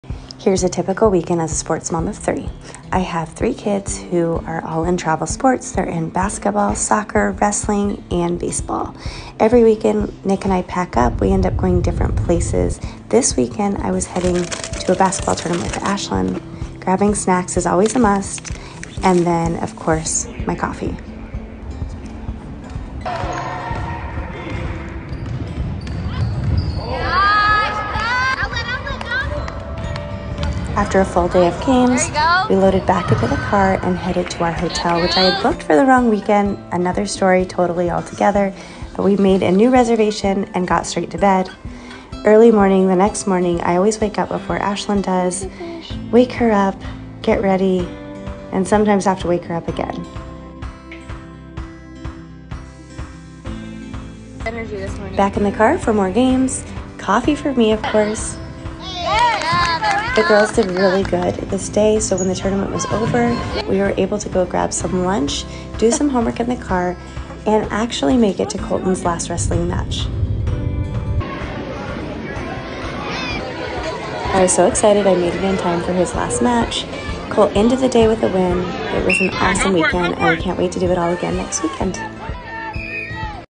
Typical weekend for a sports mom of 3
Happy Saturday!!

Can you relate? Love our busy weekends as a family of 5 and would not change the chaos and craziness for anything!!

I love sharing our lives with you!